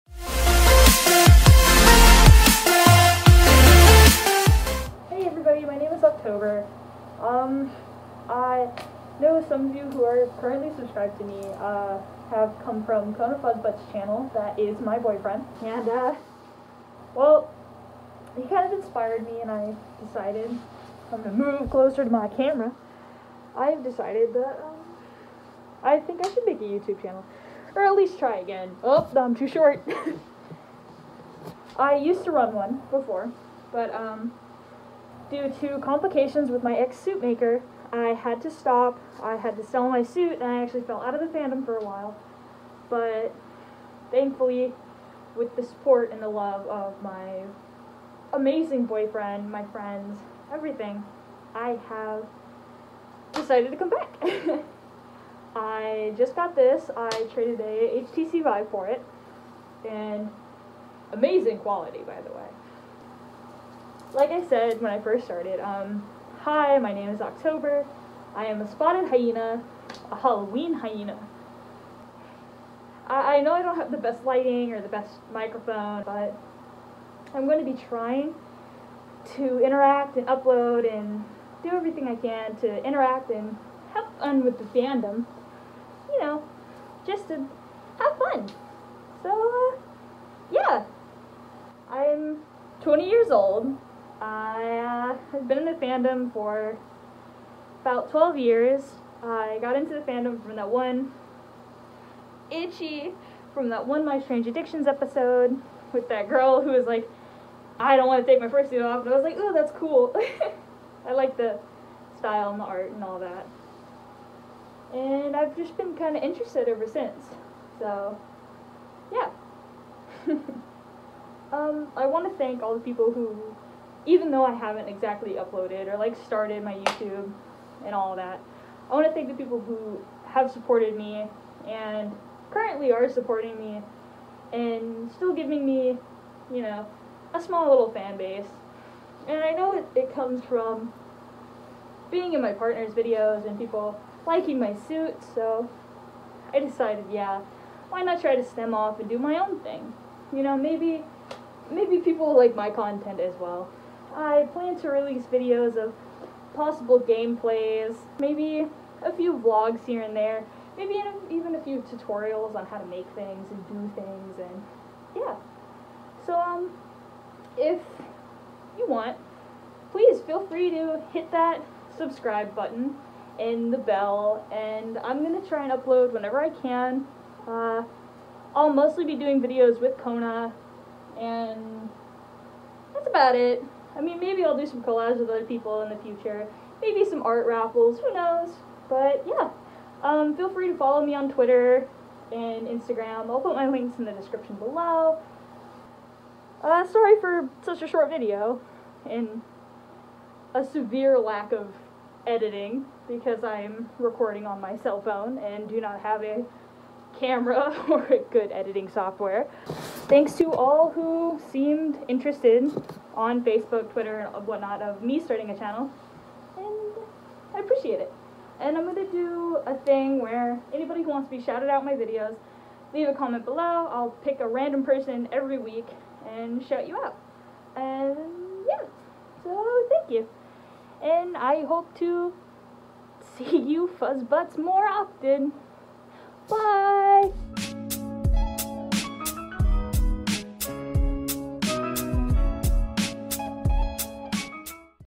Starting a furry YouTube!
Hey everyfur! I have finally decided to open up and start a new YouTube! I know this probably isn’t the best video out there with the choppy editing and bad lighting but hey! I just wanted to get something out (:

And Instagram at OctoberFuzzButt

I hope to see everyone a lot in the future!